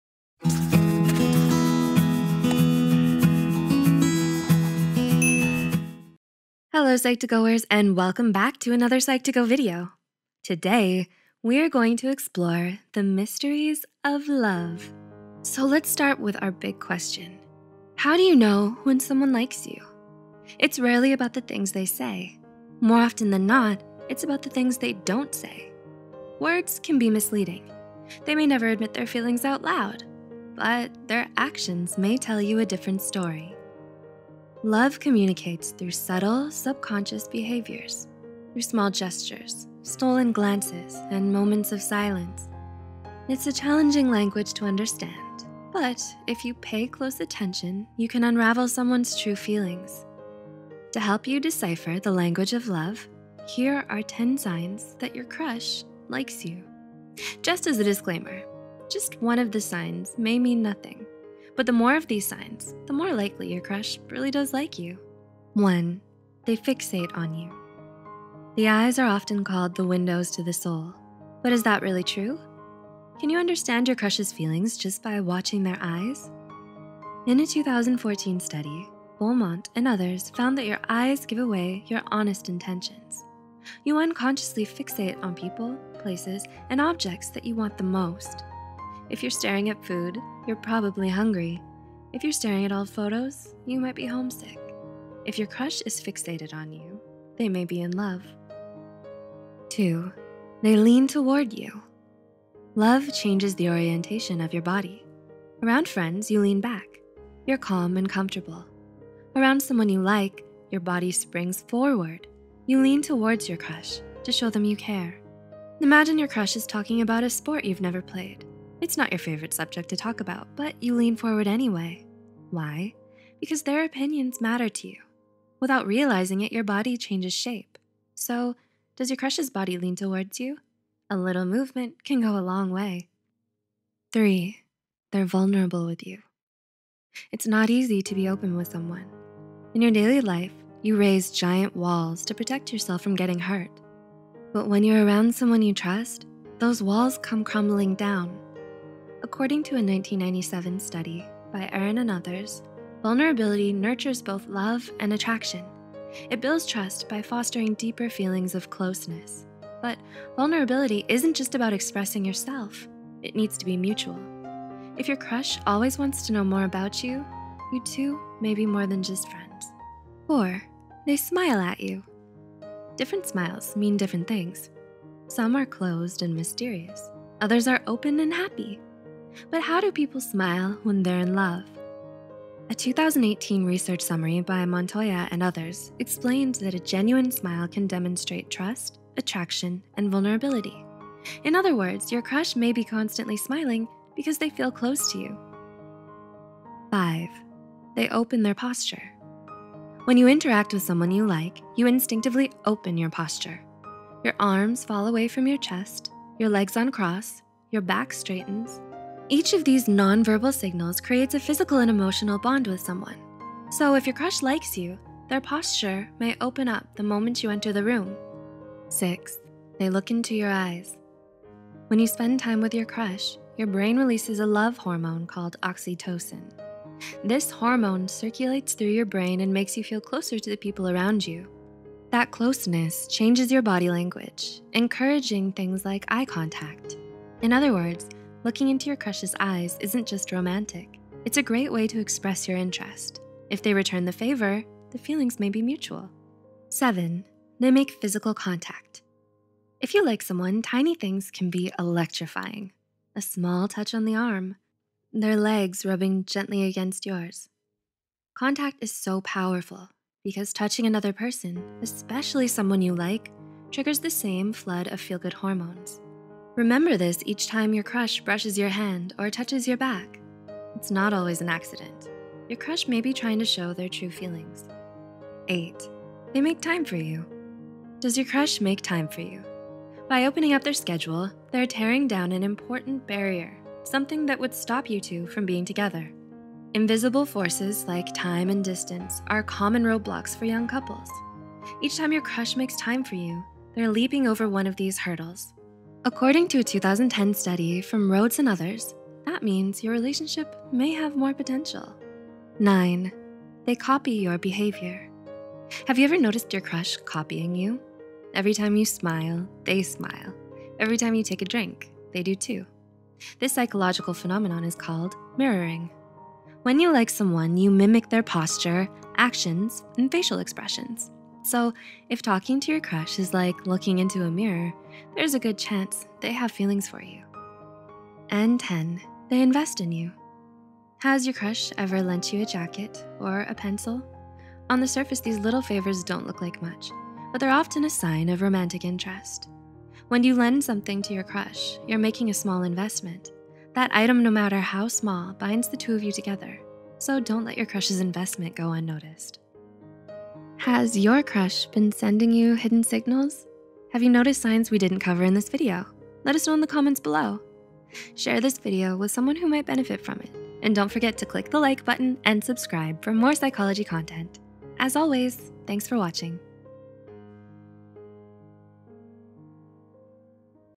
10 Signs Your Crush Likes You
What are the signs your crush likes you back? Are you looking for dating advice on how to know if your crush likes you? Do you hope to be in a relationship with your crush, but not sure if you should make the first move? Love communicates through subtle, subconscious behaviors—through body language, small gestures, stolen glances, and moments of silence. To help you decipher the language of love, here are 10 signs that your crush likes you. We also did a video on how to work with your partner or crush love style

Credits
Writer: Tristian Reed
Script Editor: Kelly Soong
VO: Amanda Silvera
Animator: Tris Canimo
YouTube Manager: Cindy Cheong